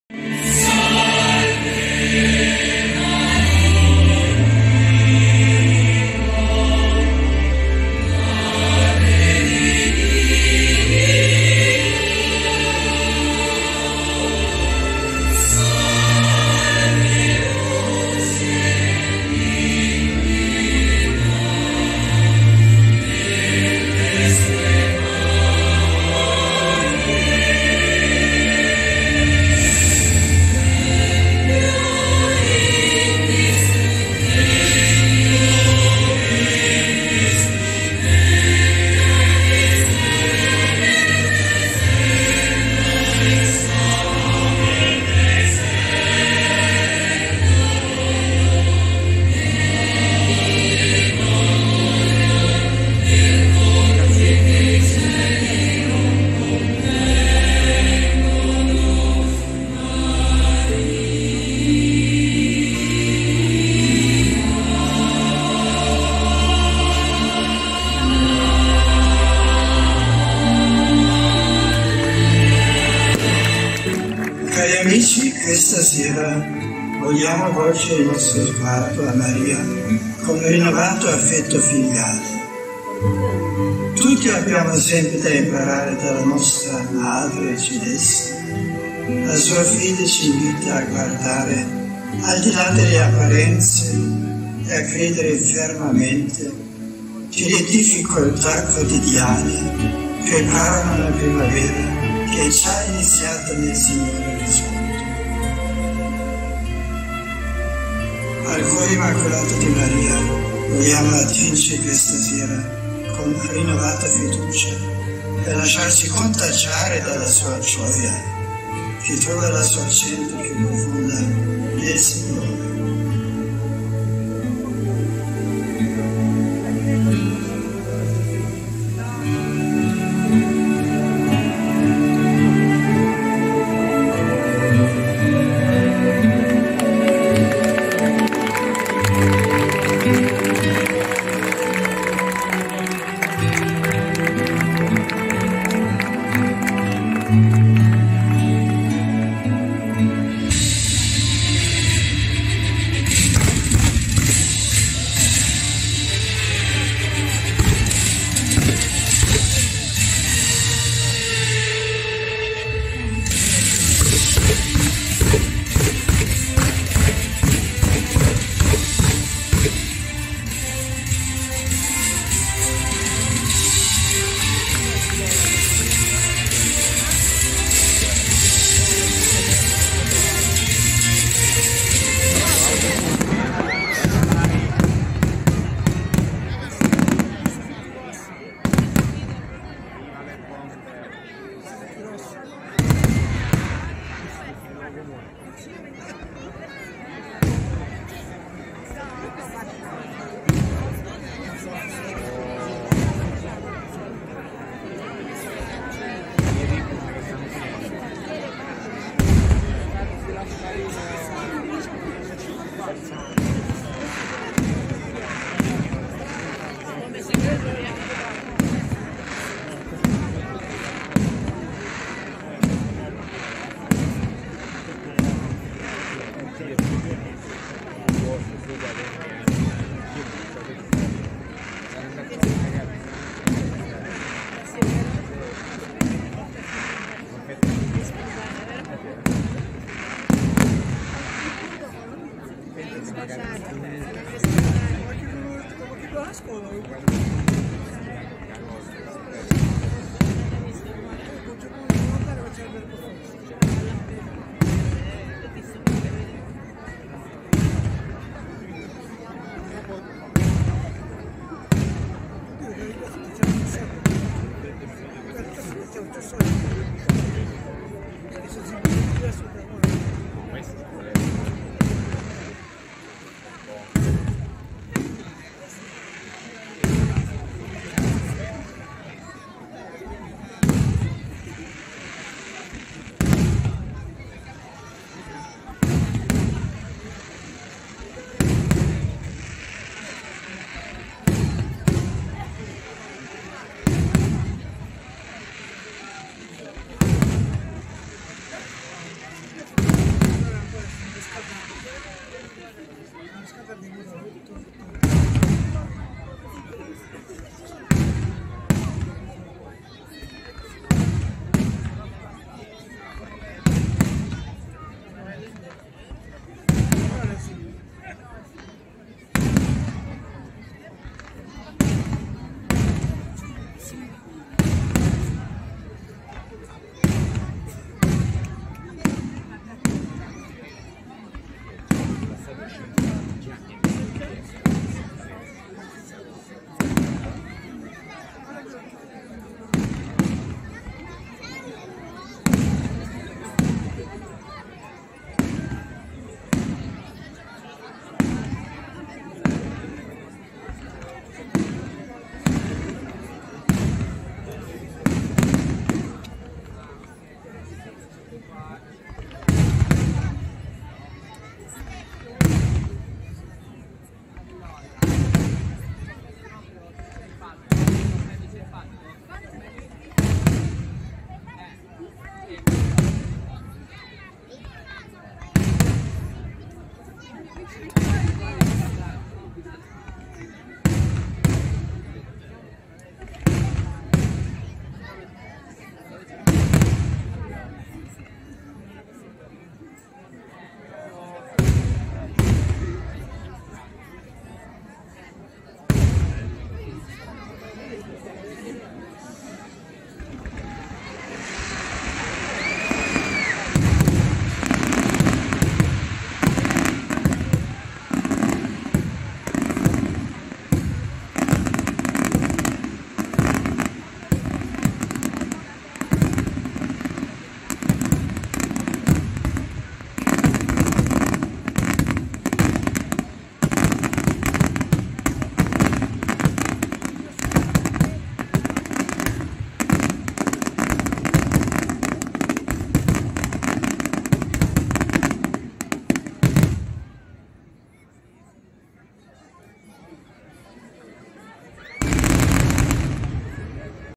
Pedara 9 Settembre 2023 Carro Partito Sant'Antonio e Spettacolo Pirotecnico Notturno Ditta FA SI MA
Vigilia dei Festeggiamenti in Onore di Maria Santissima Annunziata.
Celeste Patrona della Città di Pedara (CT).
Ingresso a Piazza Don Diego del Comitato del "Carro Partito Sant'Antonio".
E Spettacolo Pirotecnico Notturno realizzato dalla rinomata Ditta: "FA. SI. MA. FUOCHI EVENTI" dei Fratelli Spina di Santa Venerina (CT).
Piazza Don Diego di Pedara (CT), Sabato 9 Settembre 2023 - Ore 22:15.
Buona Visione!!!
Montaggio Video by Giovanni Cisafulli.